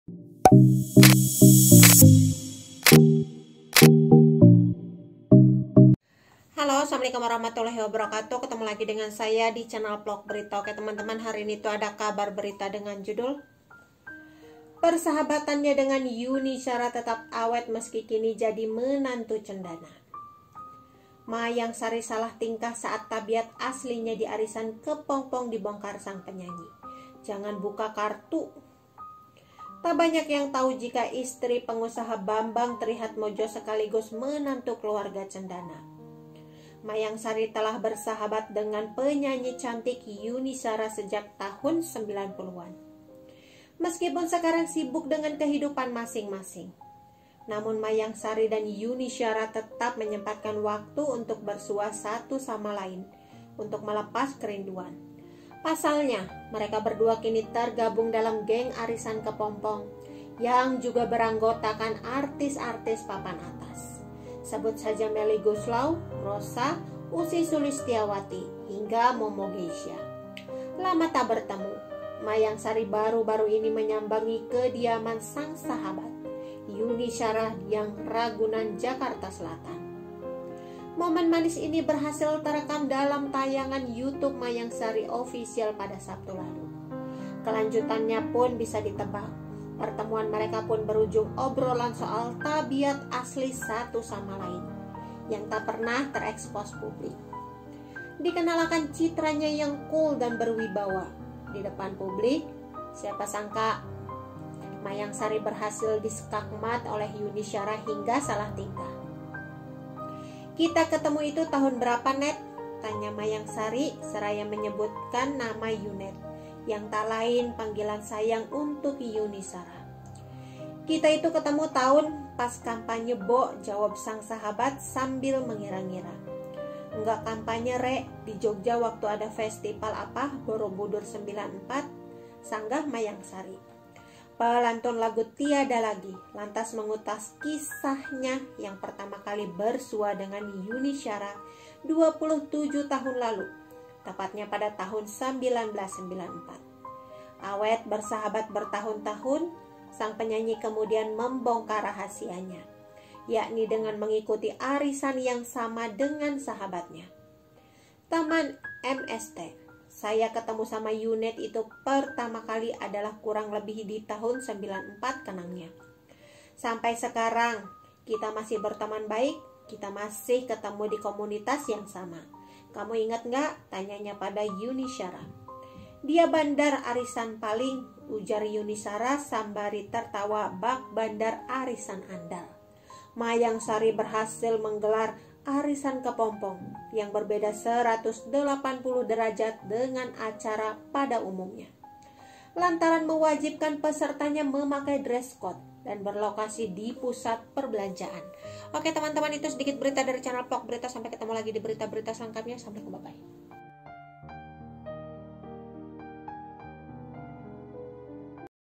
Sudah Lama Berteman Dengan Yuni Shara, Mayangsari Salah Tingkah Saat Tabiat Aslinya
Tak banyak yang tahu jika istri pengusaha Bambang Trihatmodjo sekaligus menantu Keluarga Cendana, Mayangsari telah bersahabat dekat dengan penyanyi cantik Yuni Shara sejak tahun 90-an.

Meskipun sekarang sibuk dengan kehidupan masing-masing, namun Mayangsari dan Yuni Shara tetap menyempatkan waktu untuk bersua satu sama lain untuk melepas kerinduan.